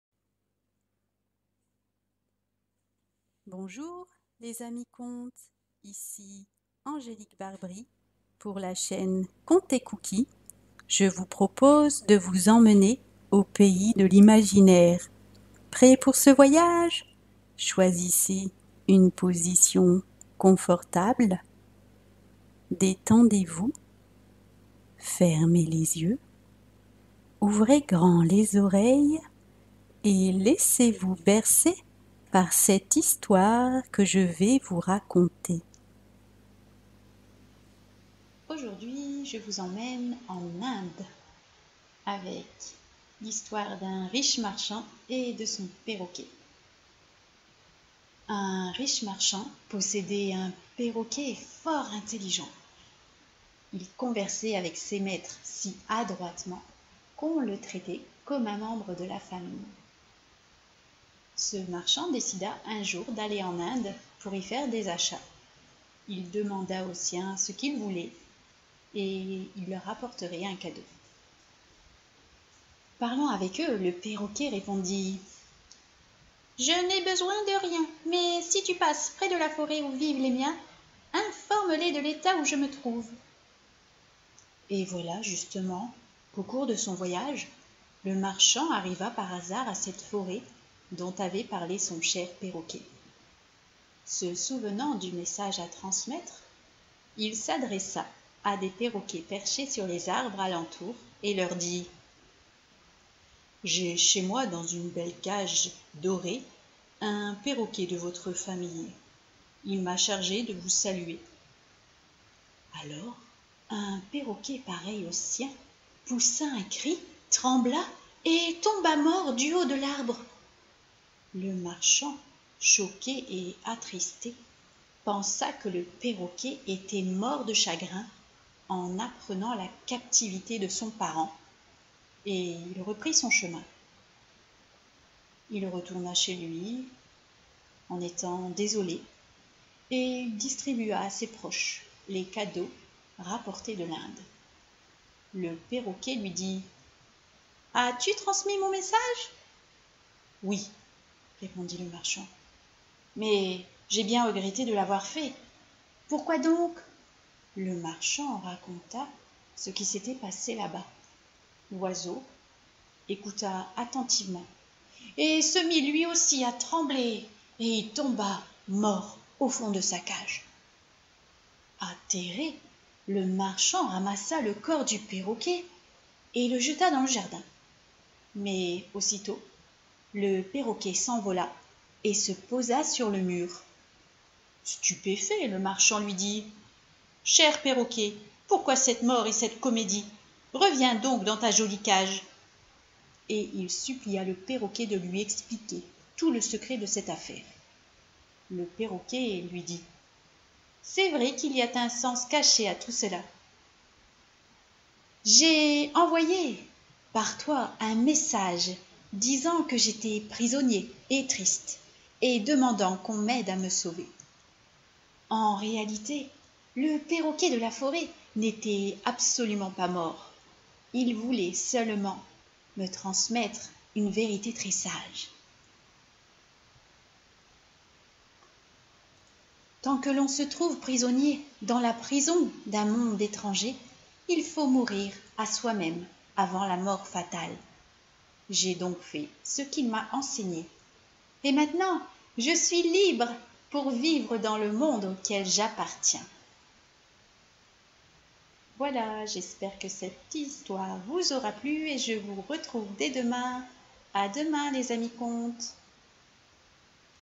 37-365 Conte inde : le riche marchand et son oiseau
Bonjour les Amis-Contes,

Aujourd'hui, je vous emmène au pays de l'imaginaire et je vous conte : le riche marchand et son oiseau.

Bonne écoute et à demain :-)

ps : illustration originale du conte par mon mari David Barbry ;-)